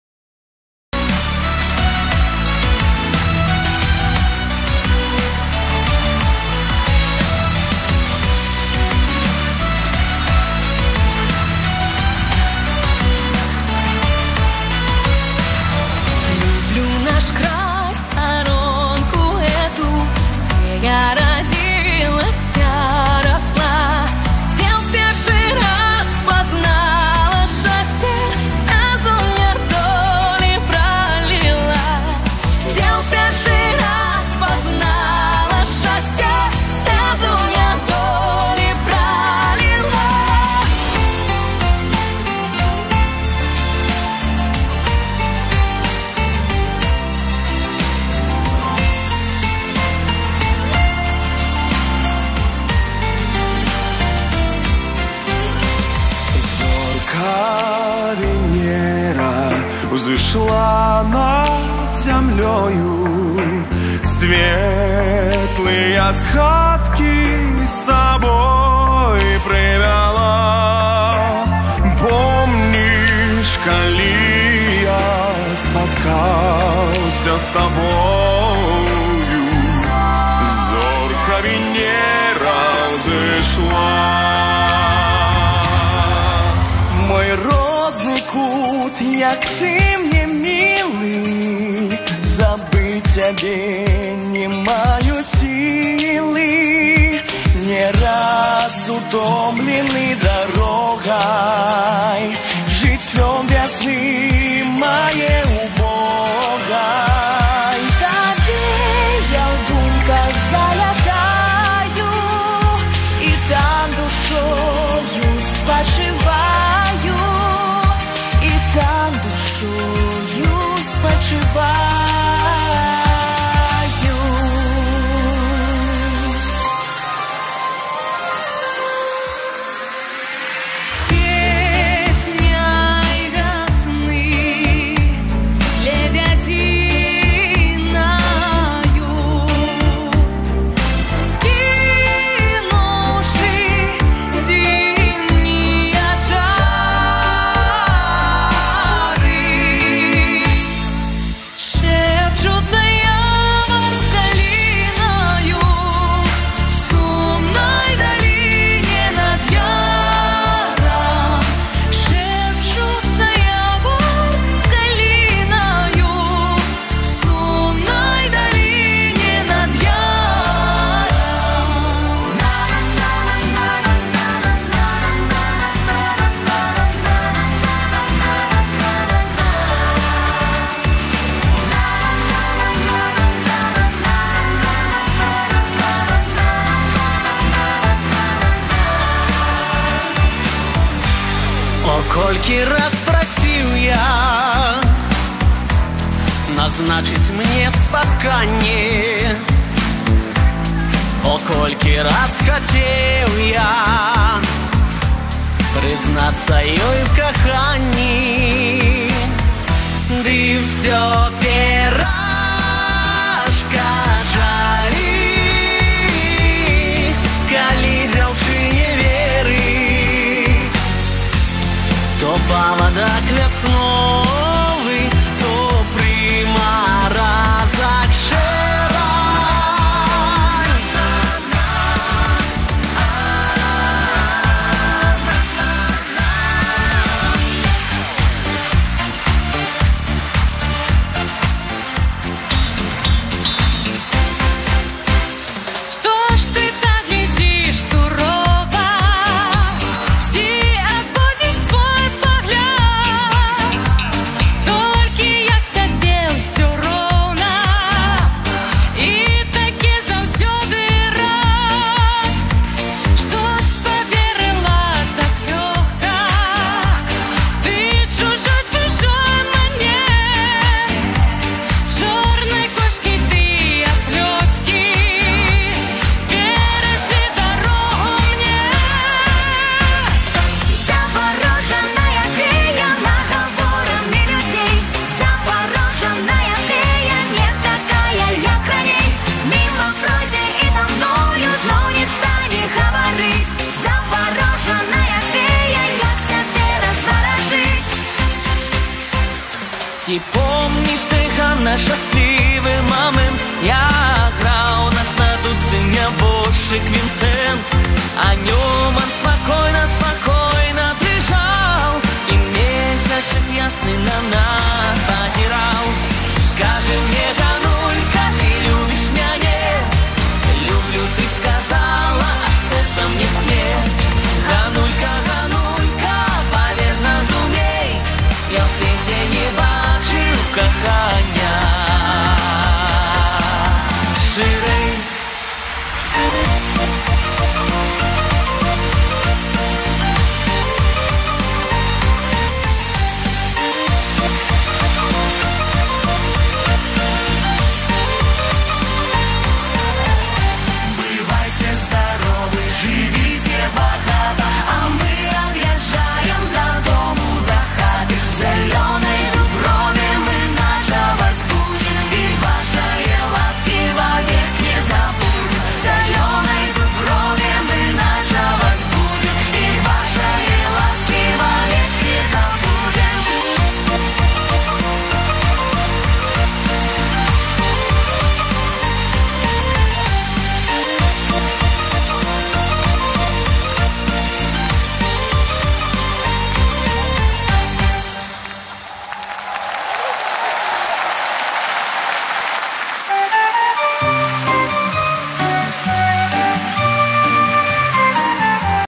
"Моваю класікаў" Концерт ко Дню народного единства 17.09.2021 Минск
"Моваю класікаў" – Никита Камадей, Даниил Мышковец, Алексей Будько, Влад Жук, Алена Синицына, Анна Трубецкая, Наталья Ширко, Елена Кузнецова